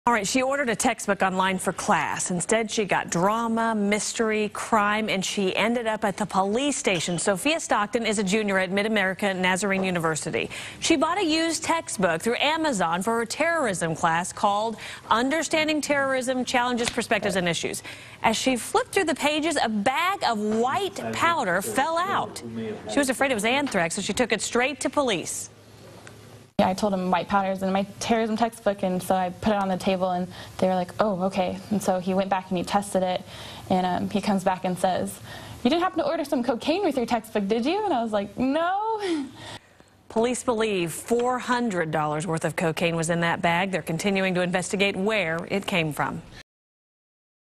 Cocaine found in textbook ordered through Amazon
She ordered a textbook online for class -- instead she got drama, mystery and crime and ended up at the police station.